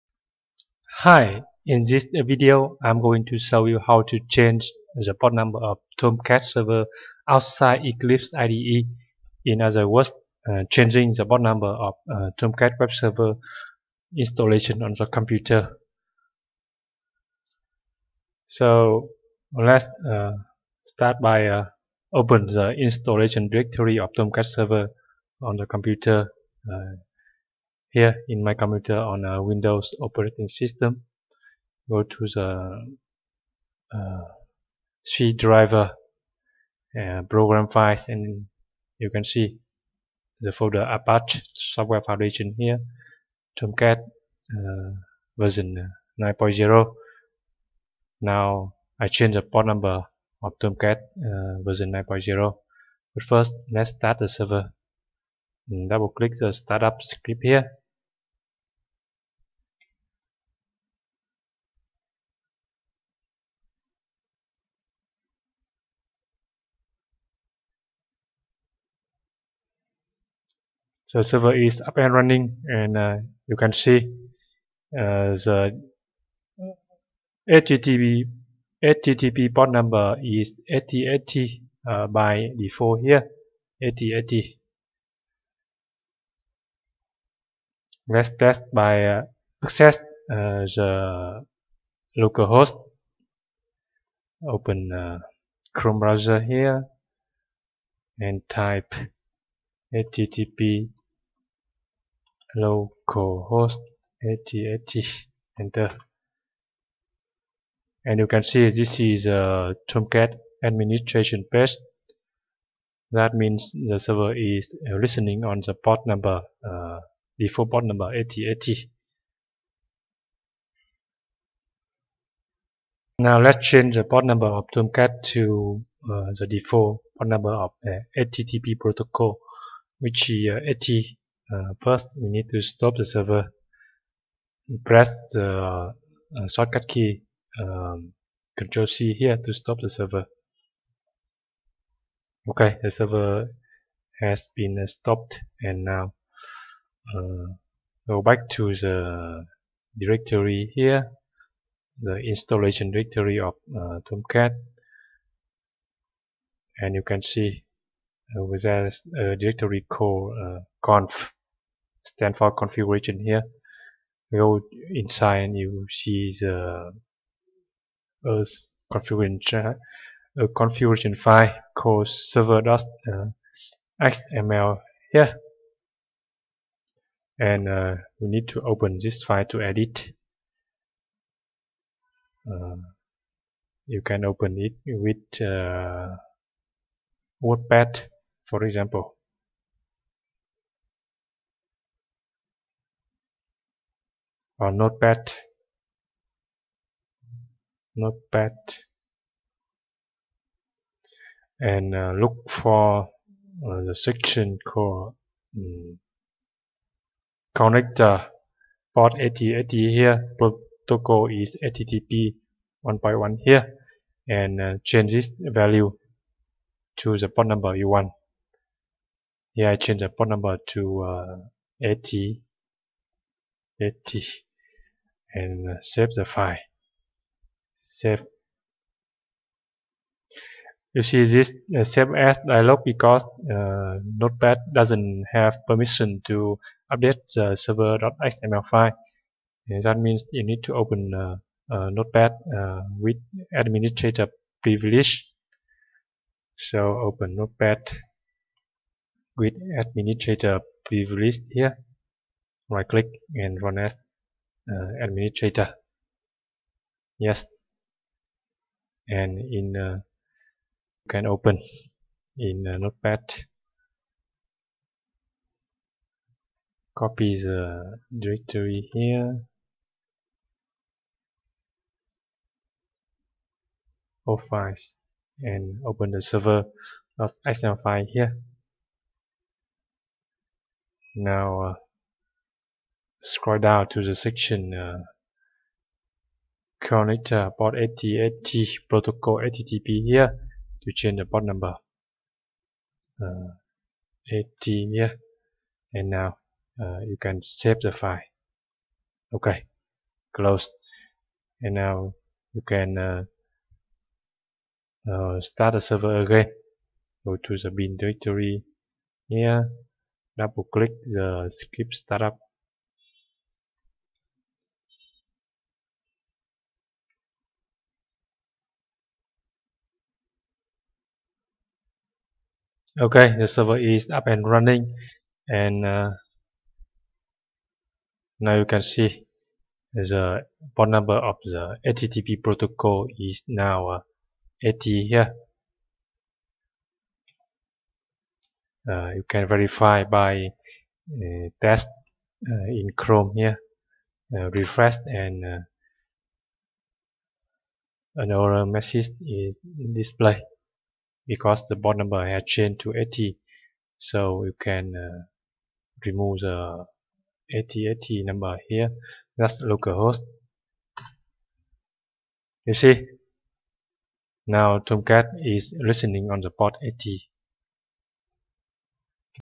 How to change port number of Tomcat outside Eclipse
How to modify HTTP port number of Tomcat installation on your computer - Not in Eclipse IDE. NOTE: changing port number by this way may not affect the port number of Tomcat in Eclipse IDE because Eclipse makes its own copy of Tomcat's configuration files.